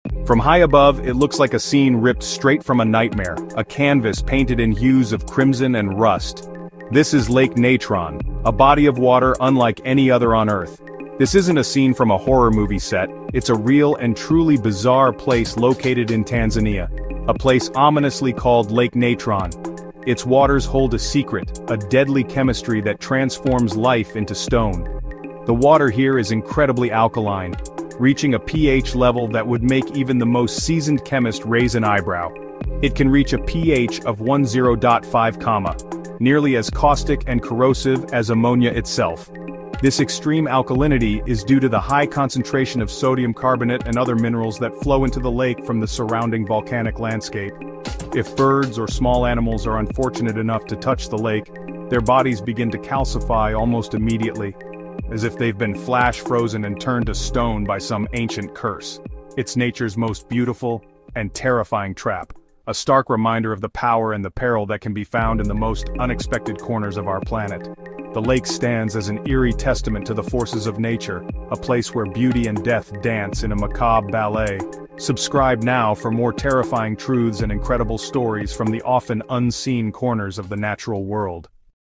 Lake of Death 💀: Where Animals Turn to Stone!
In Northern Tanzania lies a haunting red lake — Lake Natron.
Its water is so alkaline, it can literally preserve animal bodies like statues.
Many birds and bats that fall into it are found calcified along the shore.
But why is it so deadly? And could anything survive there?
Watch to uncover the terrifying science behind the “Lake of Death.”
🧪 pH: Up to 10.5
📍 Location: Northern Tanzania
☠️ Effect: Mummifies creatures on contact